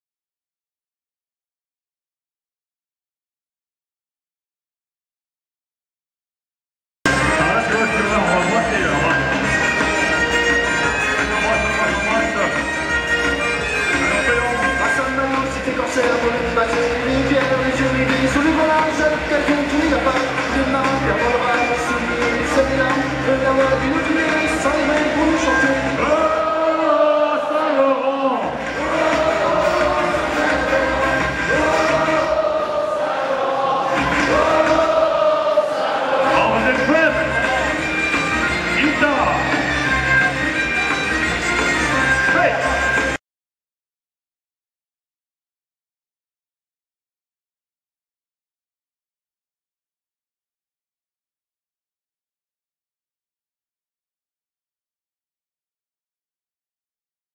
St Laurent 2011- Rassemblement
Rassemblement des St Laurent de France en Val de Loire. 2011.Par LES JARDINS D'OLIVIER à Beaugency